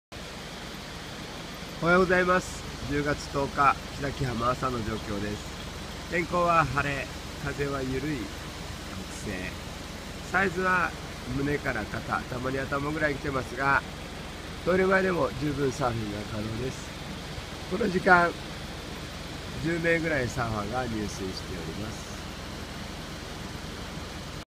2013.10.10 朝の木崎浜波情報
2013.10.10 朝の木崎浜波情報です！！

天気：晴れ／気温：23℃／水温：25.7℃
風：西1m/s／うねり：南東1.2m 7.2s
ブレイク：ダラダラしたブレイク
ウェット：スプリング
WAX：ワーム
中潮：満潮9:45,20:56、干潮15:18
日の出：6:14、日の入:17:49